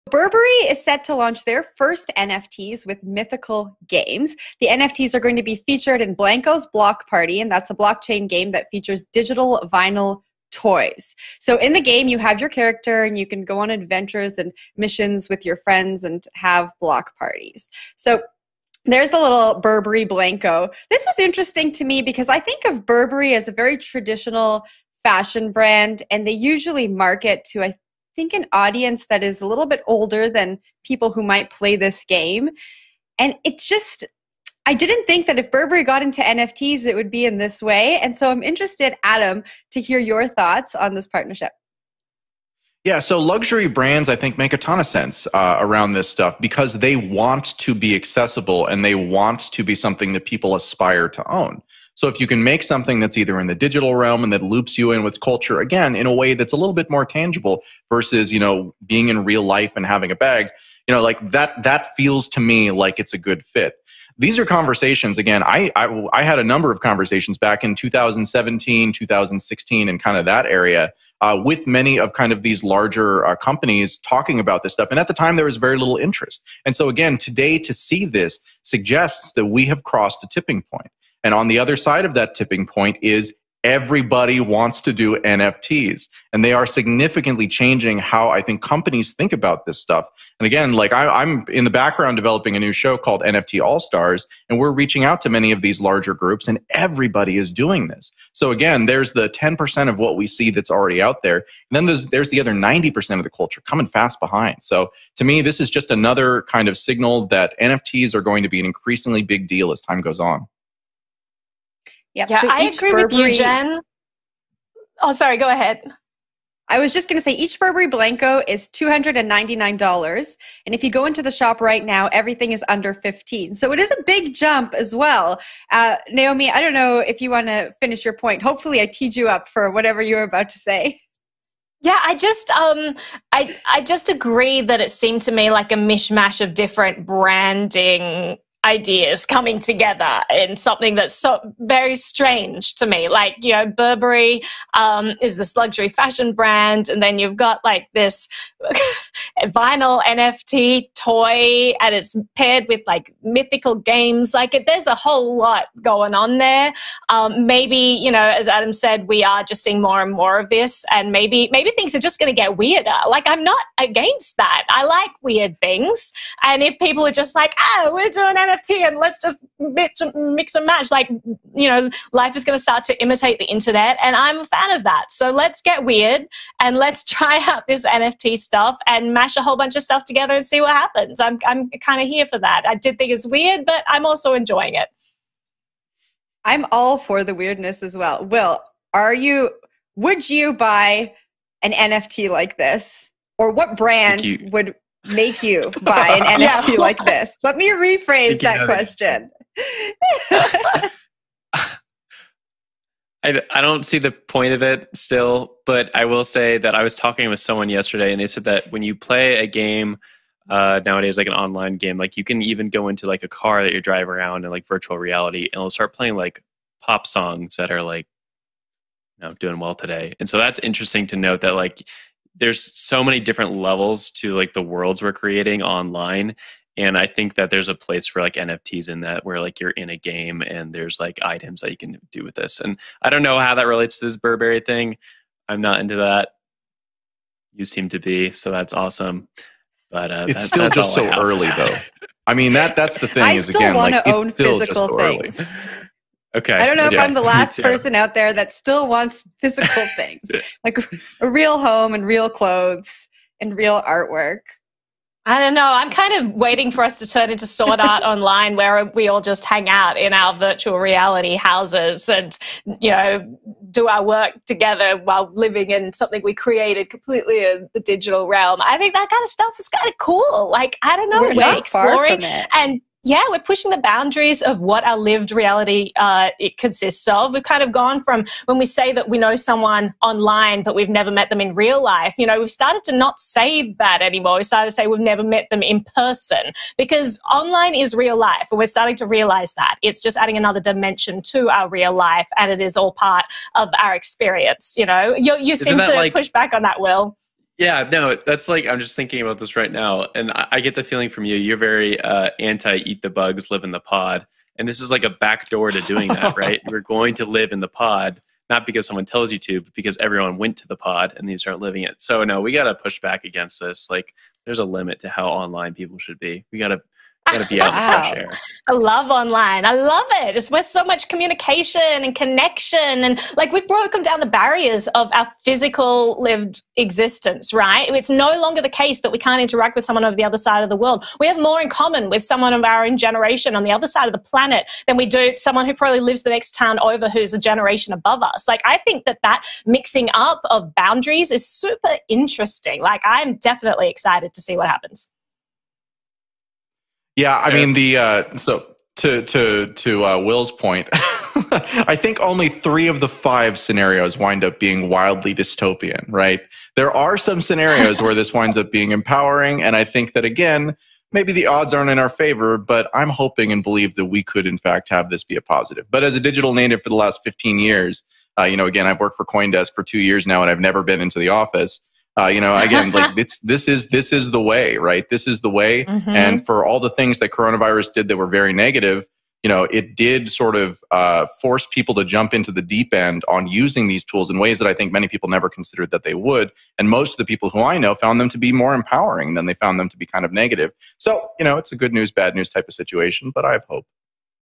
UK Fashion House Burberry Releases First NFTs in Collaboration With Mythical Games
Iconic British luxury fashion brand Burberry has launched its first non-fungible token (NFT) collection in partnership with Mythical Games, featuring limited-edition product drops via Blankos Block Party, a game with digital vinyl toys known as Blankos that live on the blockchain. "This is just another kind of signal that NFTs are going to be an increasingly big deal as time goes on," host Adam B. Levine said.